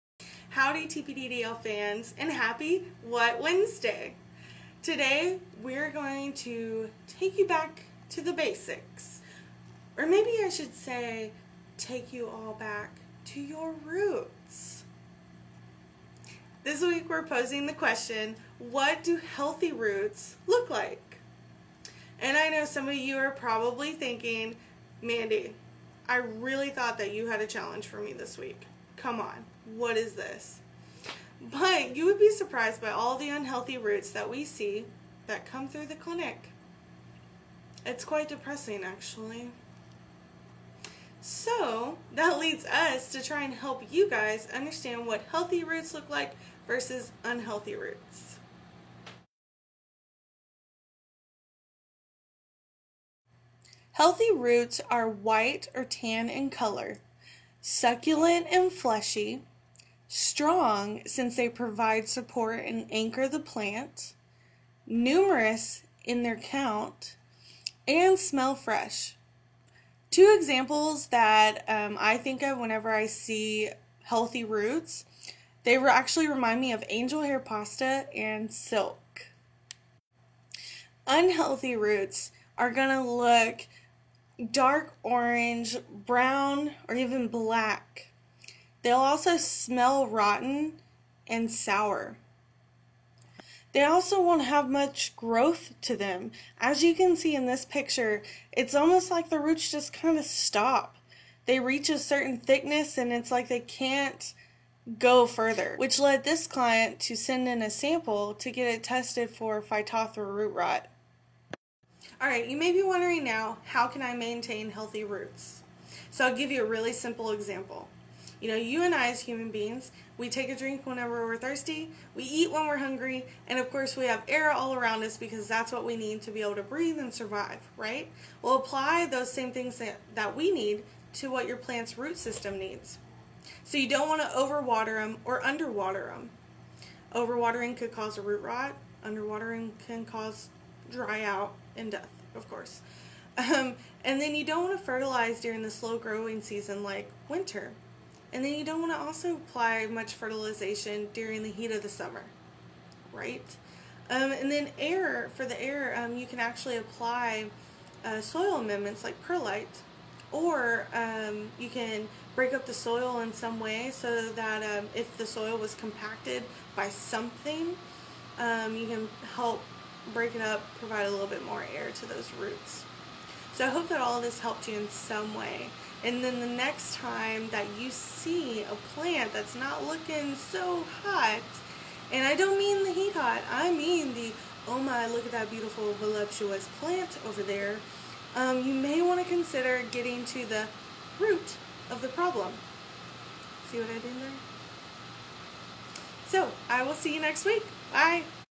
WHAT Wednesday: Healthy vs. Unhealthy Roots
Mandy informs you of the differences between what your plant's root system look like when they are healthy vs. unhealthy and what you can do about it!